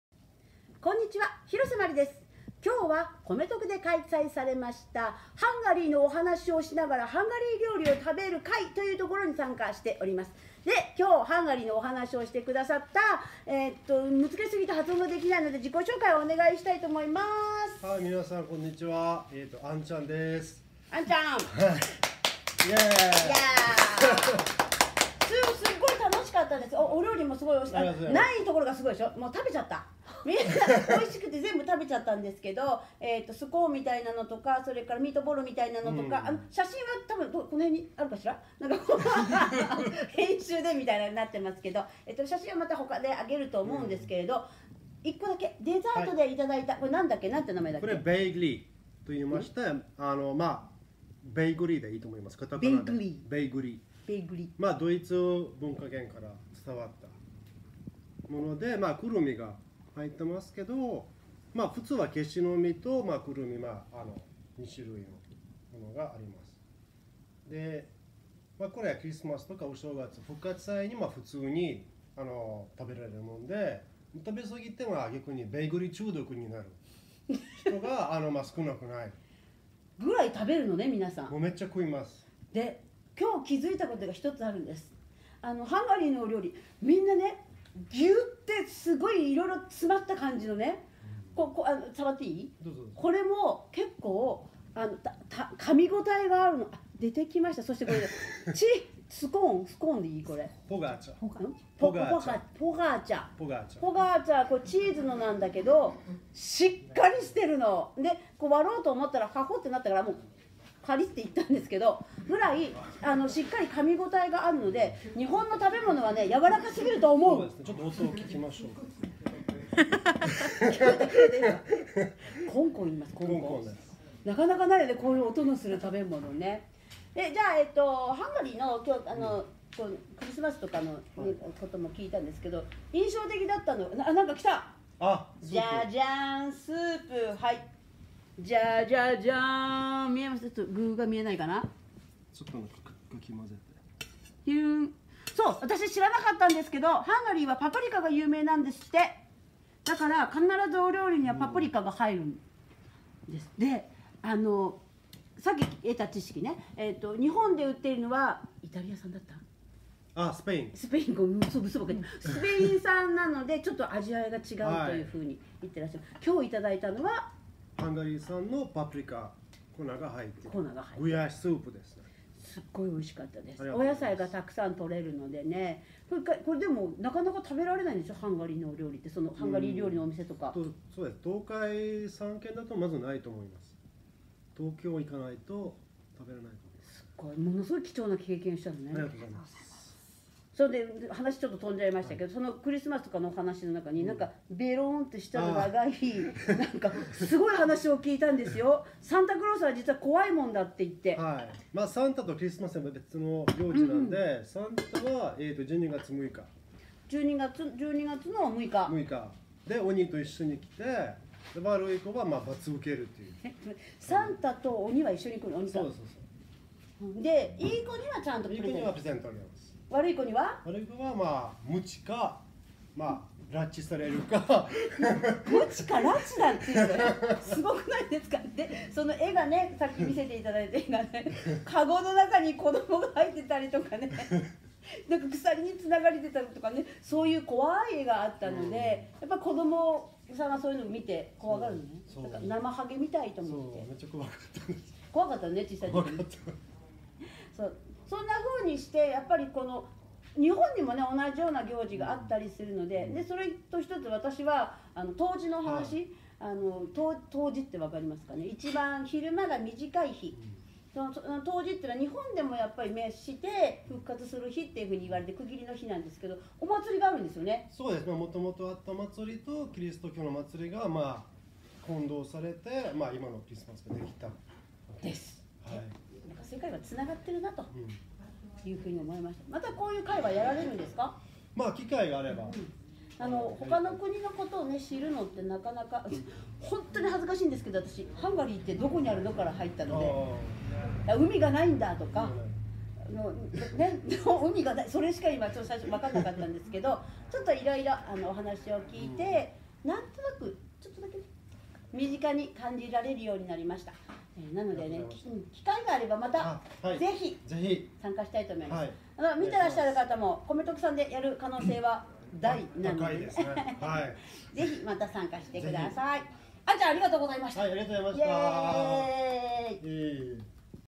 ハンガリーのお話を聞きました。
20181224
尾張旭市にある
come to kuで開催された
「ハンガリーのお話をしながらハンガリー料理を食べる会」に参加しました。

お話をしてくださった、
アンチャンにお話を聞きました。